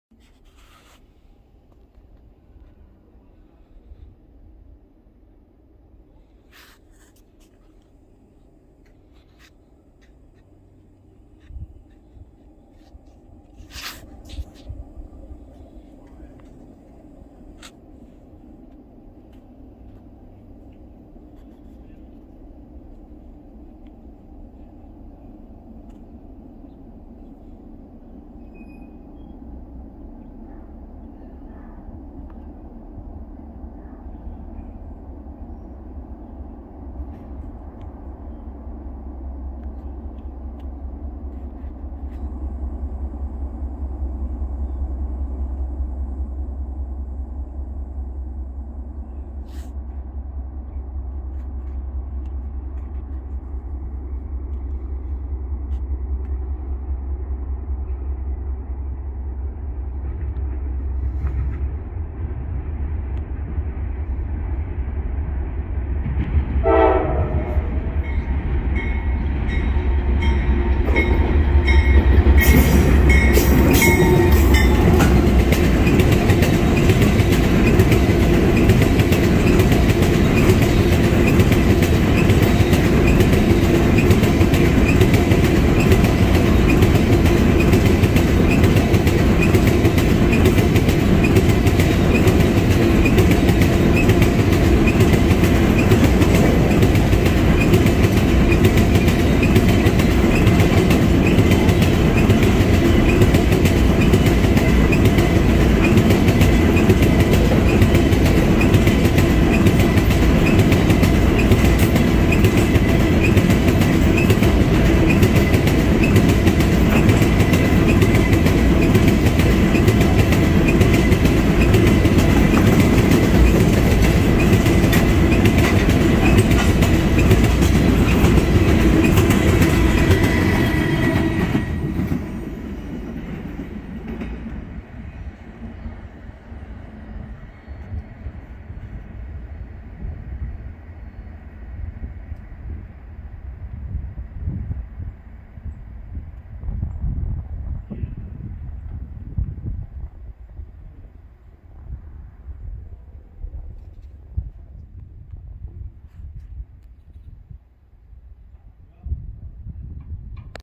Norfolk Southern 4512 leads an Amtrak ballast special at Lewistown, PA (11/26/2022)
61M